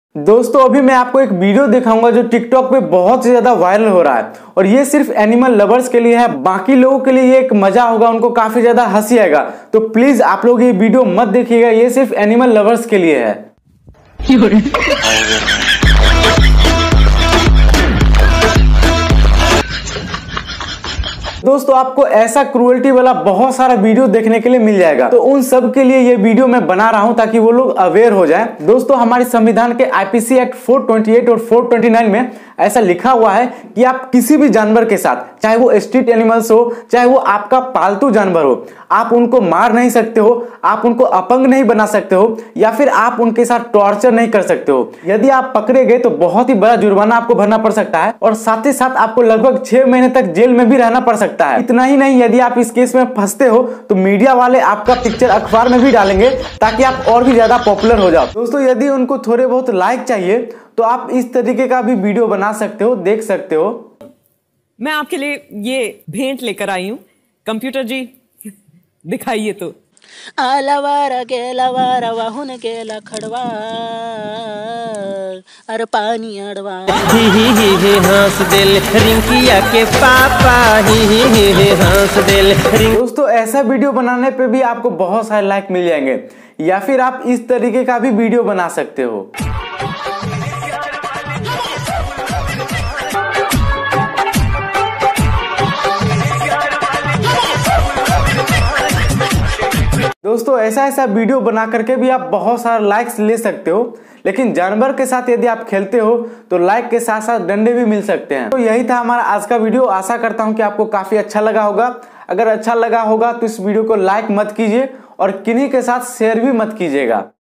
Tiktok वालों ने इस कुत्तों के साथ क्या किया - अंत तक जरूर देखें 😮😮
This is a tiktok viral video of dog. I shared my openion. What do you think write in the comment.